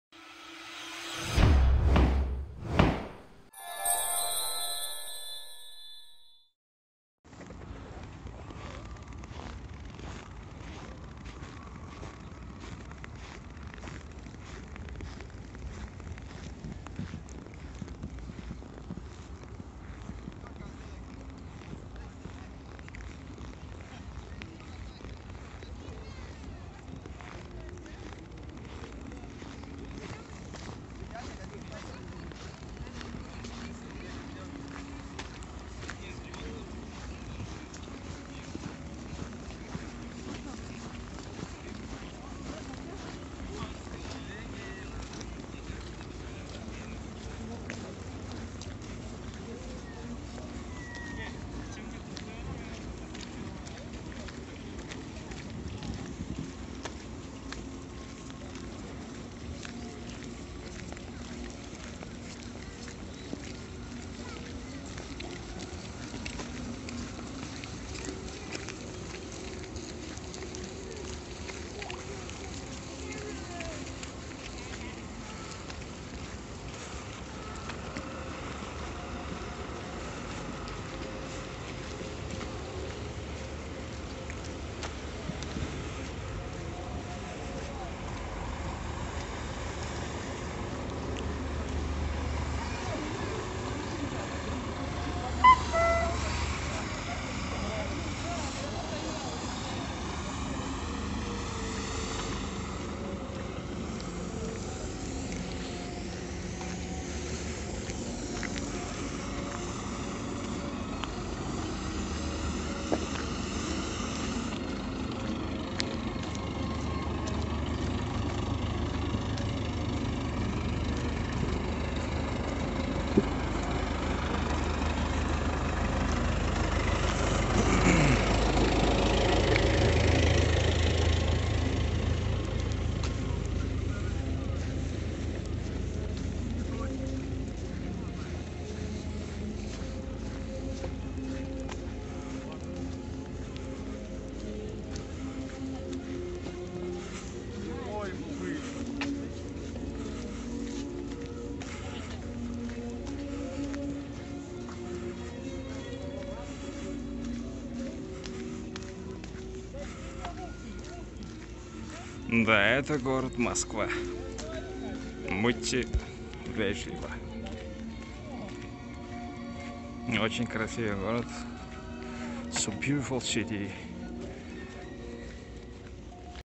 Winter in the Moscow (VDNKh) || ВДНХ
VDNKh is located in Ostankinsky District of Moscow, less than a kilometer from Ostankino Tower. It is served by VDNKh subway station, as well as by Moscow Monorail. Cosmonauts Alley and the Worker and Kolkhoz Woman statue are situated just outside the main entrance to VDNKh. It also borders Moscow Botanical Garden and a smaller Ostankino Park [ru], and in recent years the three parks served as a united park complex.